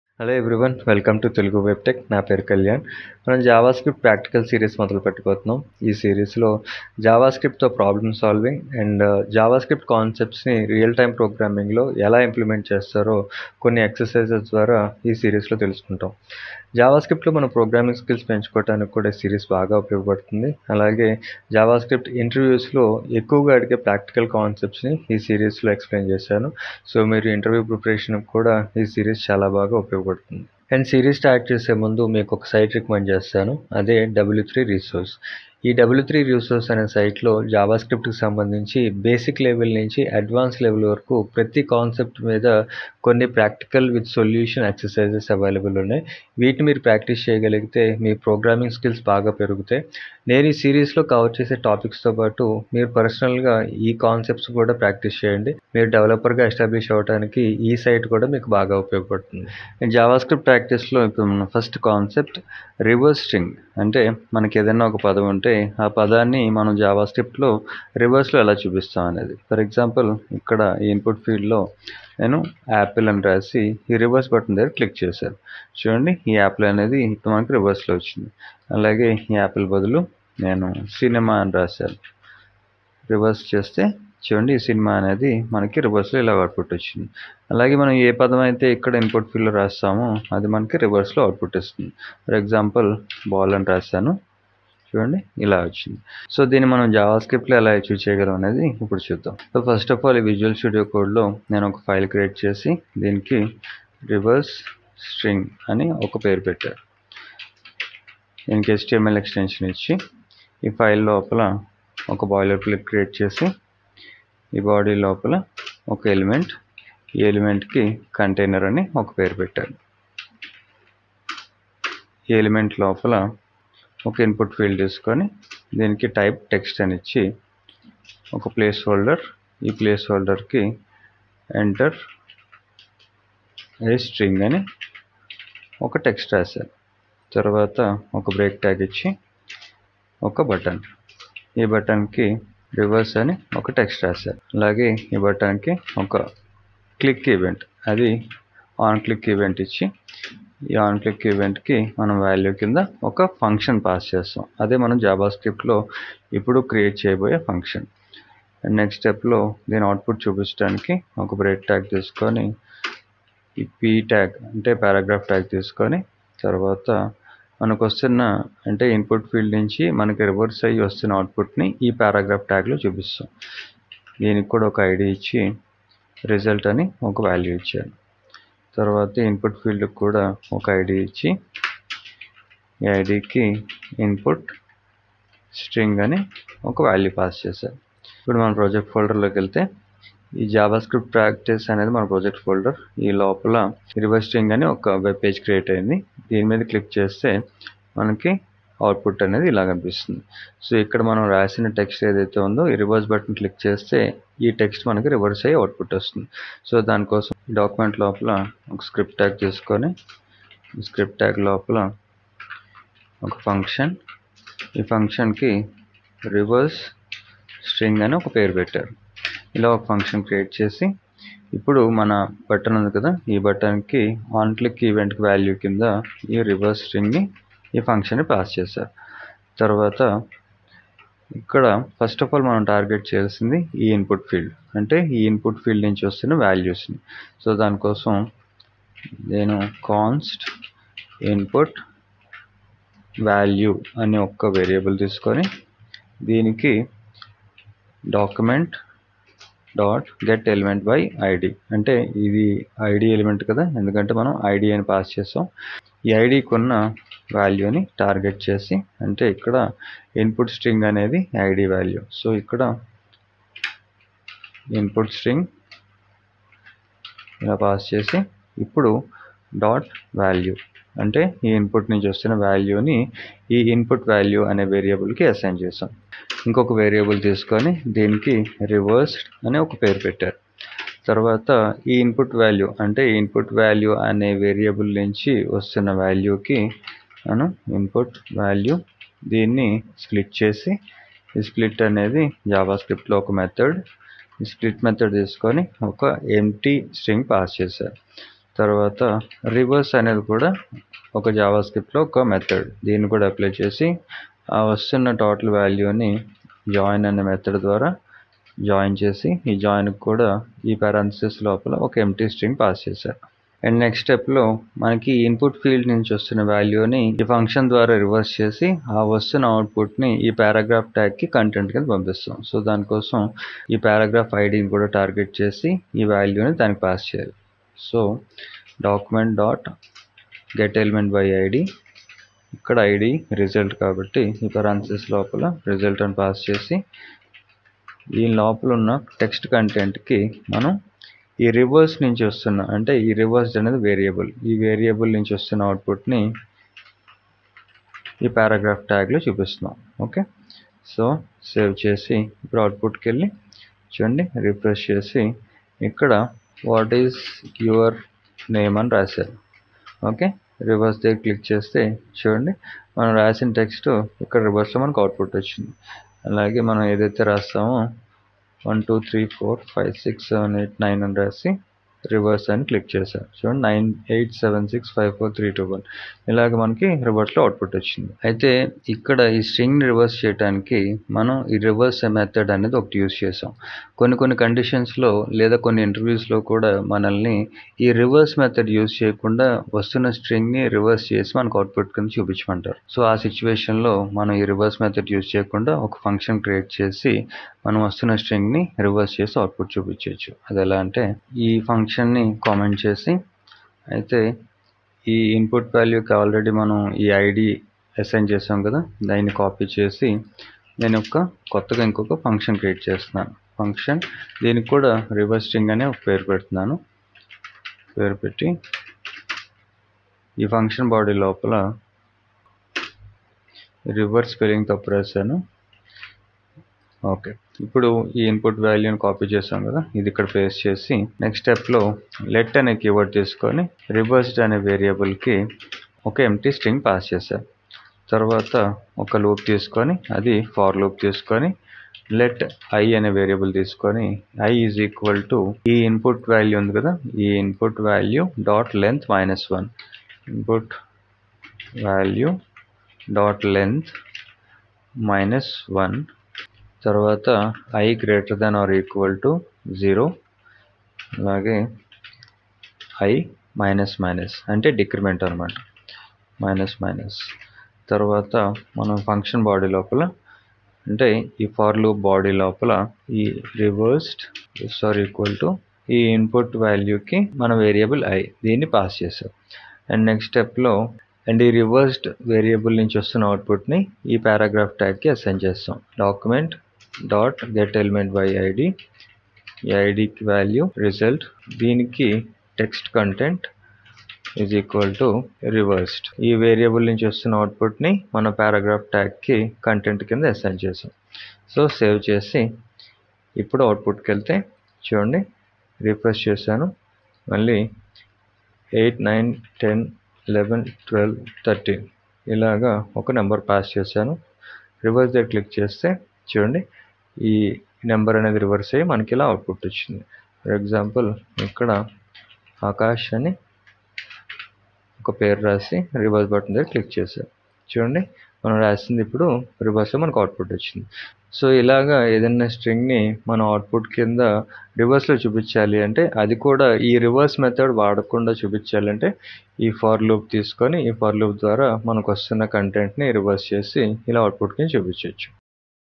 #1. JavaScript Practical's || String Reverse with JavaScript
This is the series of JavaScript Practical's, in this video you will learn how to reverse a string with JavaScript.